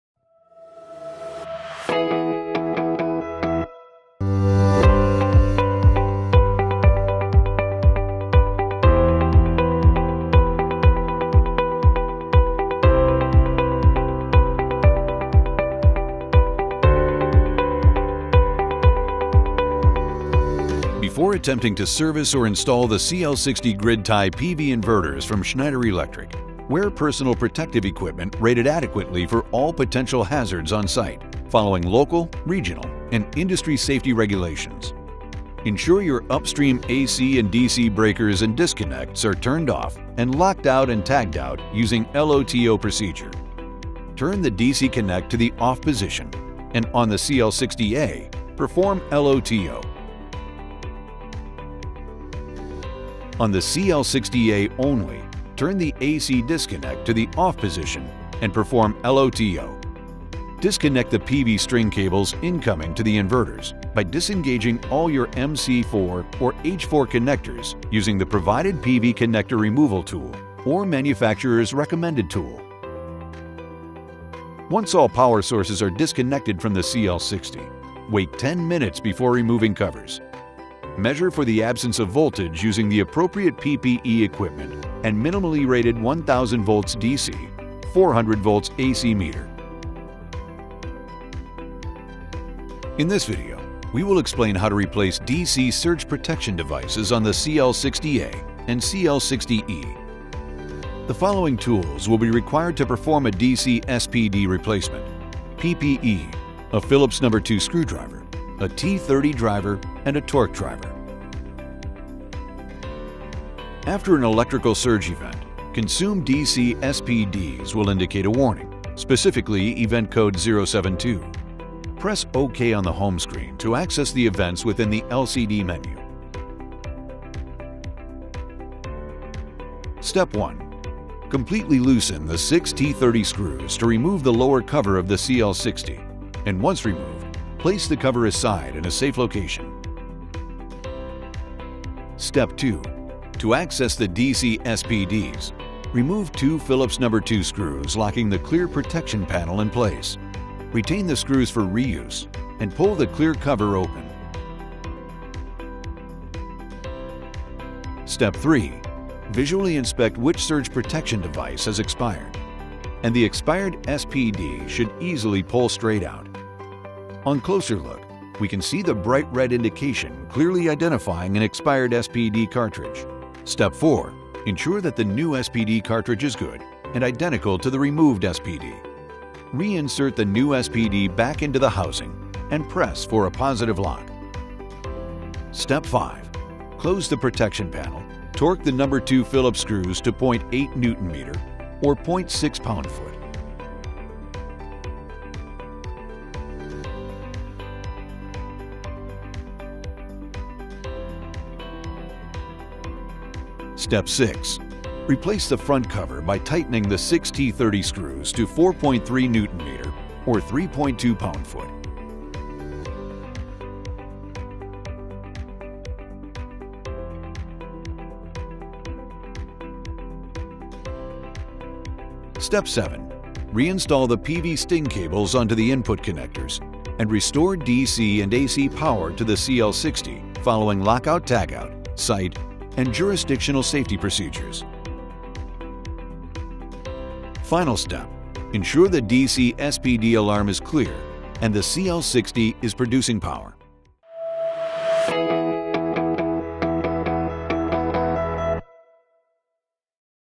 Conext CL-60 DC SPD Replacement
The Conext CL-60 DC SPD Replacement video explains how to diagnose and replace expended DC Surge Protection Devices on the Conext CL-60A and CL-60E PV grid-tie inverters from Schneider Electric.